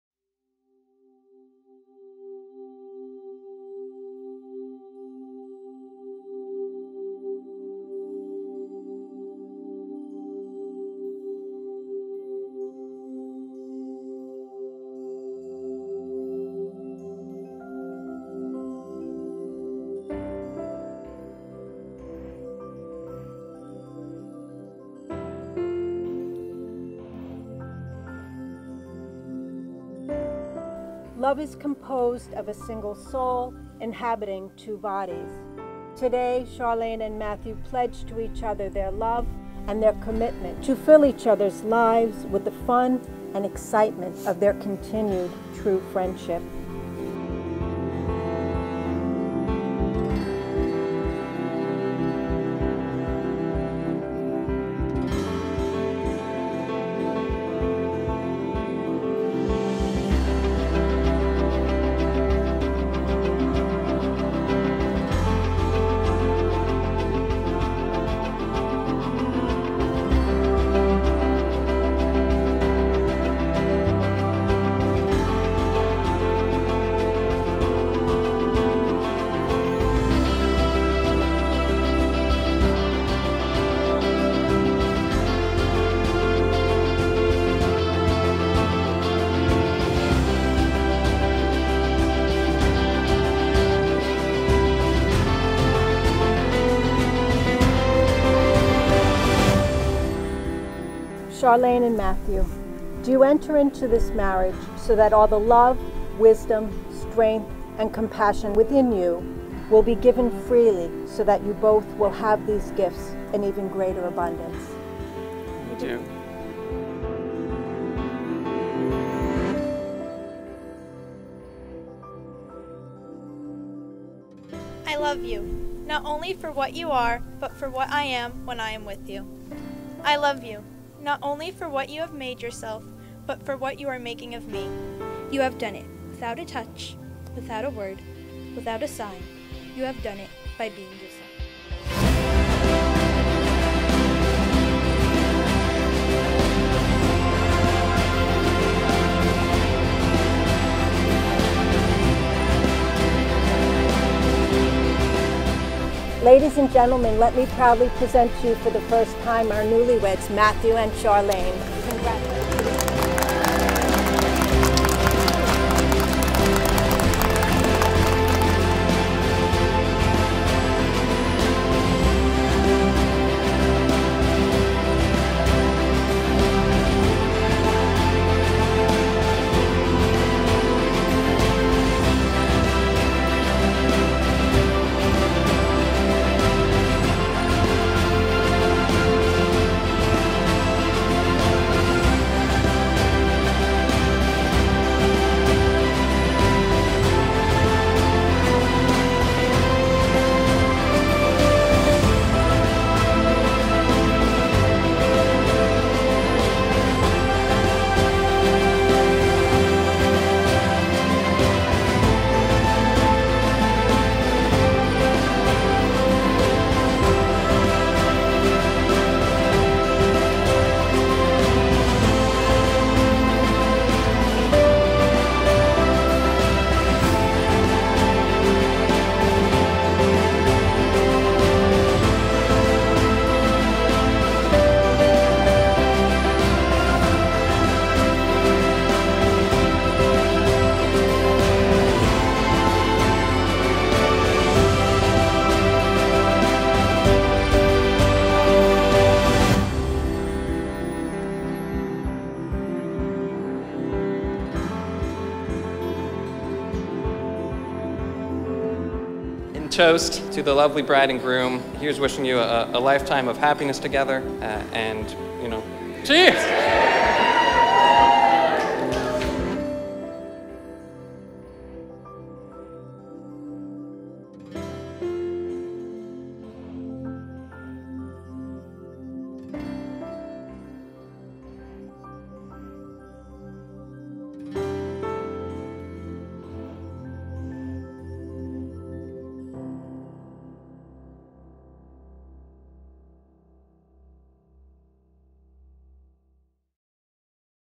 Saltwater Farm Vineyard Wedding Video - CT Wedding Videographer - NST Pictures
Love this film? Get pricing and availability for your big day

Charlayne & Matthew
NST Pictures Wedding Cinematography
Trailer Film
Basic Collection + Feature Film Add-on

Reception Venue: Saltwater Farm Vineyard
Stonington, CT